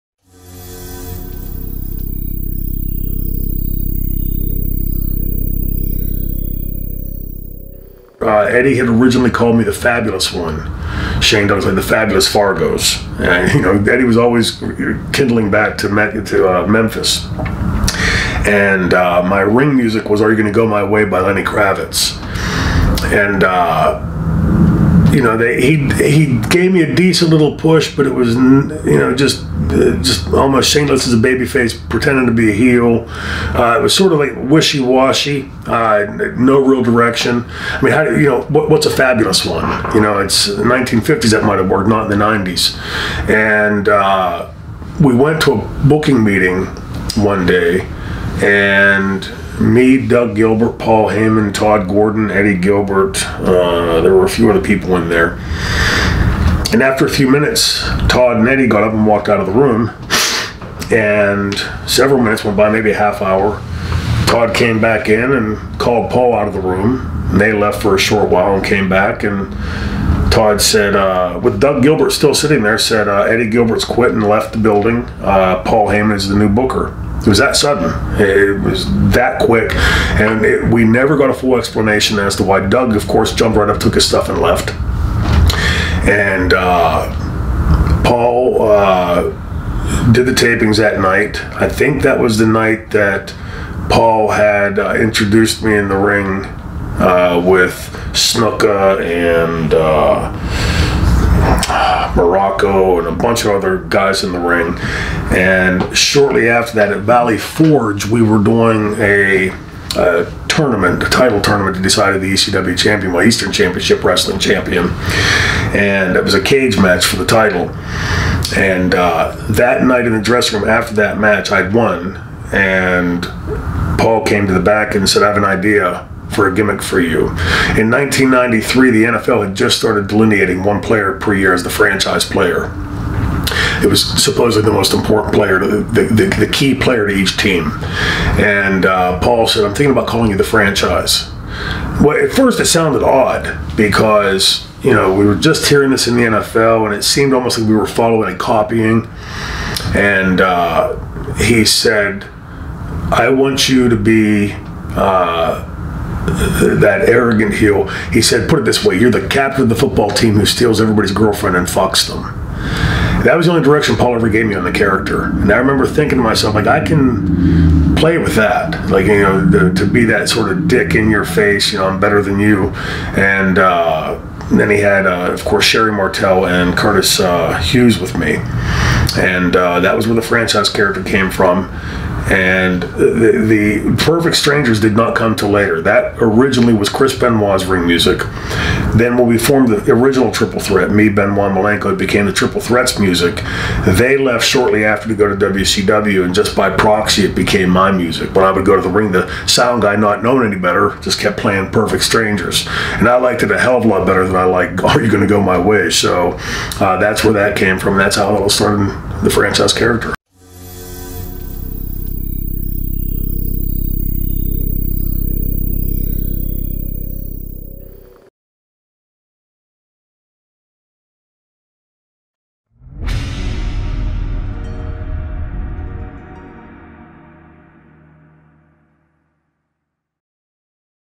Shane Douglas on becoming "The Franchise"
Former ECW World Champion, WWE & WCW Superstar "The Franchise" Shane Douglas explain how is Extreme Championship Wrestling character was developed.